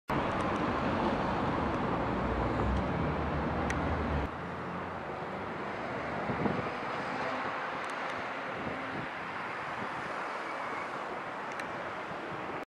EDDIE STOBART LYNNE EDNA IN GLOUCESTER 180119
Eddie Stobart Scania L8004 Lynne Edna at Argos ,St Oswalds,Gloucester